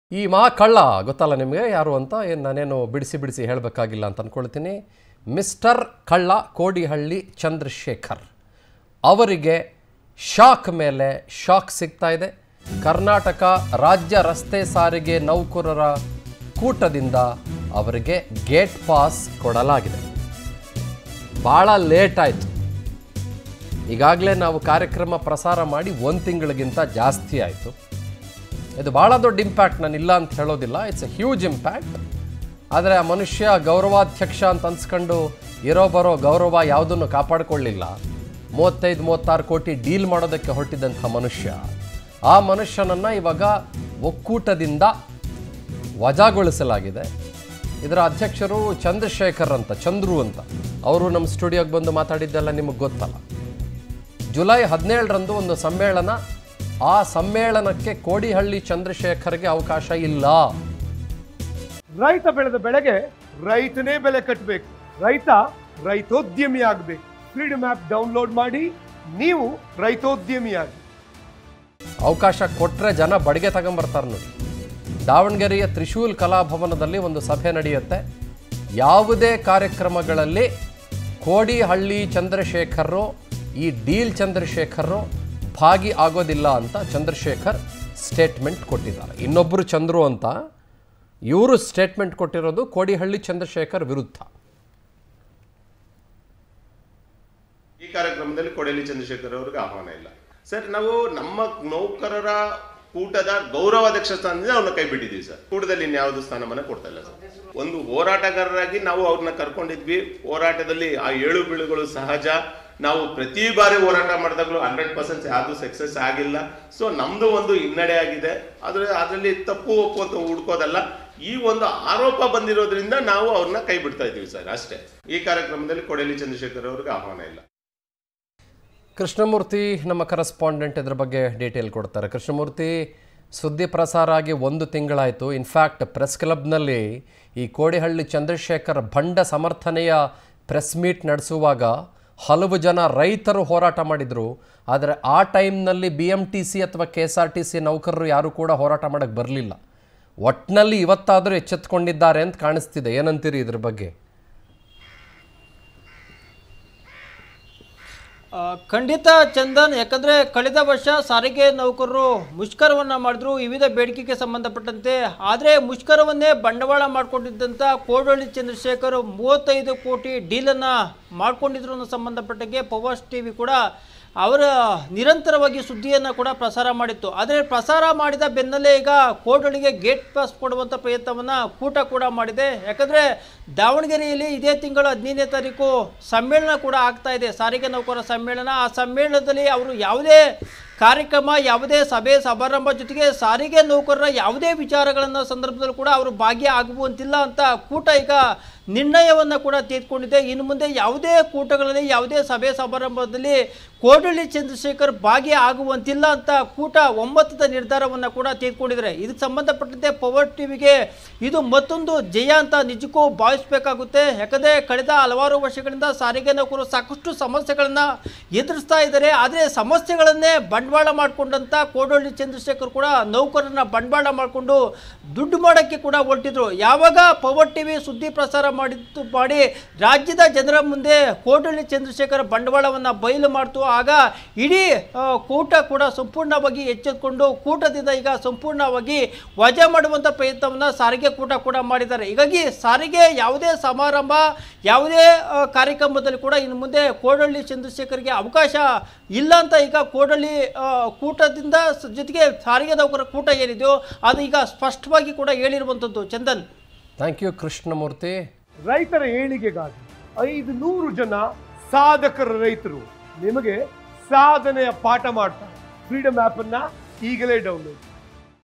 ರಾಜ್ಯ ರಸ್ತೆ ಸಾರಿಗೆ ನೌಕರರ ಕೂಟದಿಂದ ಕೋಡಿಹಳ್ಳಿ​ಗೆ ಗೇಟ್​ಪಾಸ್ | Kodihalli Chandrashekar | Power TV
More Updates Keep Following Us